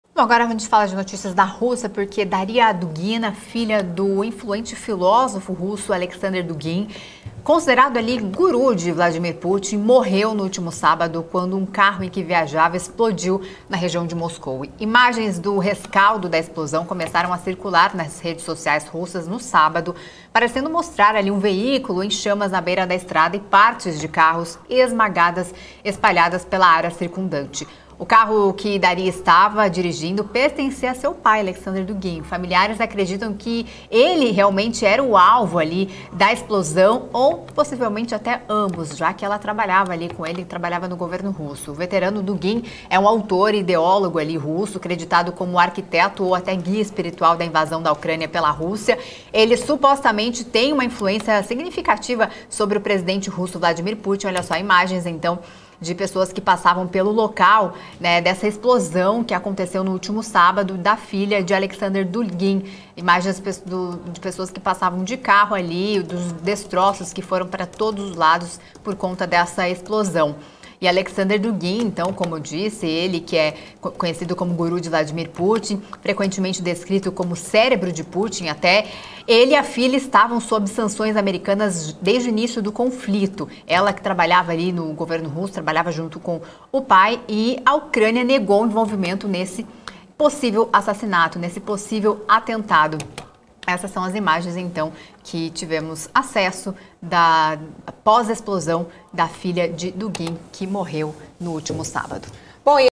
FILHA DE "GURU" DE PUTIN MORRE EM EXPLOSÃO DE CARRO
Filha de Alexander Dugina, creditado como o guru de Vladimir Putin é morta em explosão de carro. Há suspeitas de cidadã ucraniana envolvida na morte.